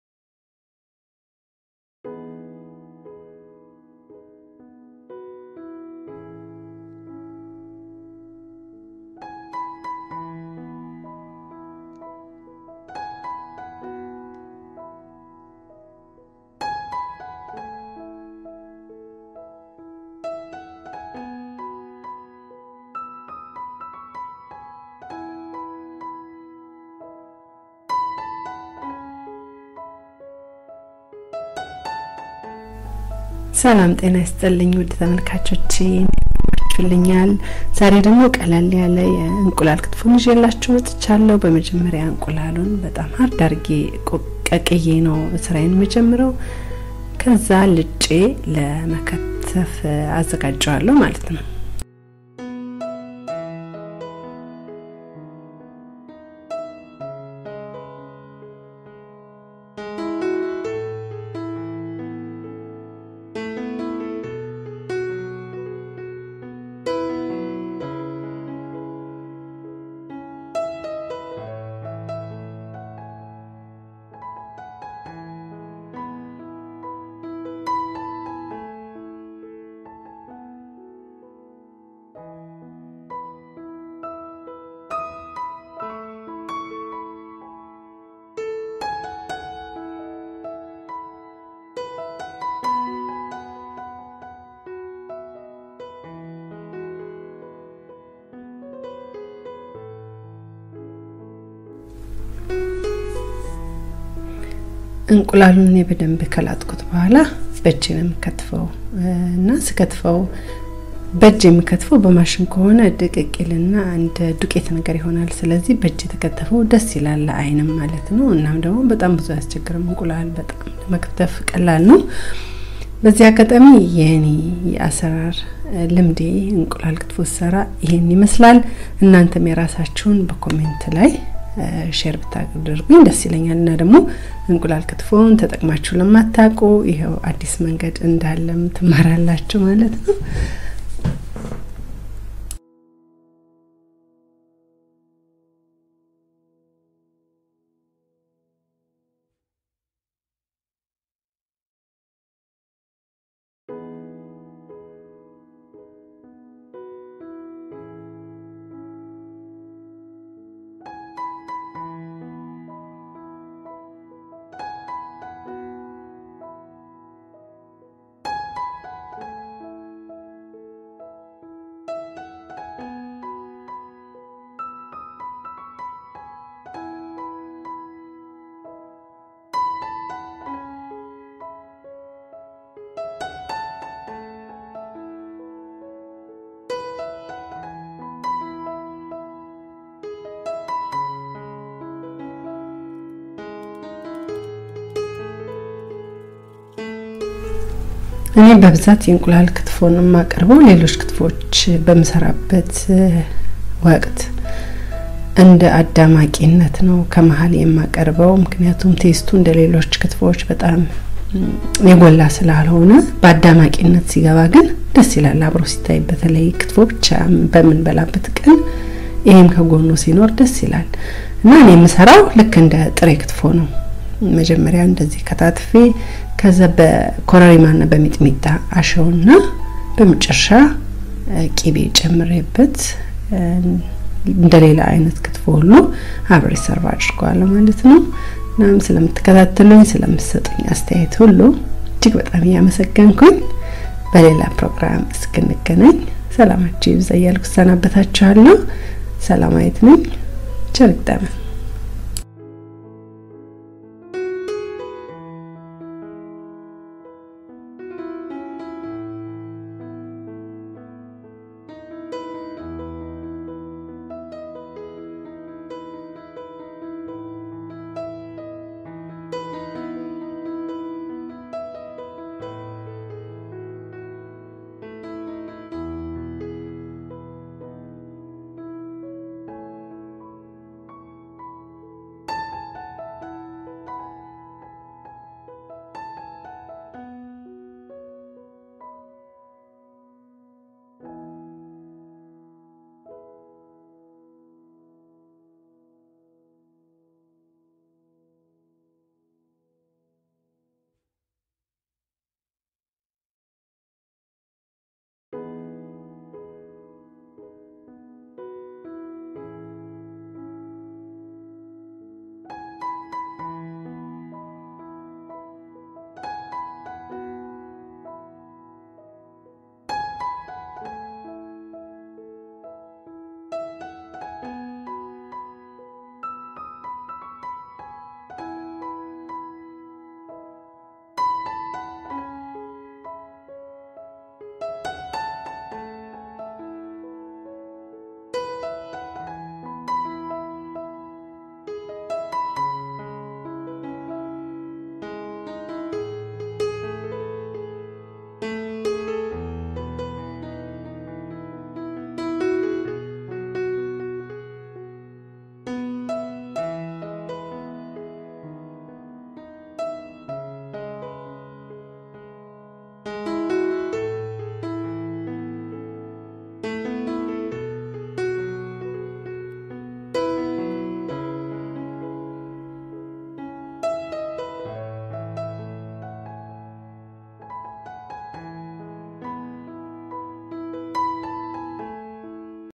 Kitfo with egg ልዩ የእንቁላል ክትፎ Ethiopien cusine Äthiopisch Kochen
ingredients
6 eggs
Nitirkibe
Mitimita
Korerima

Zutaten
6 Eier
Nitirkibe
Mitimita
Korerima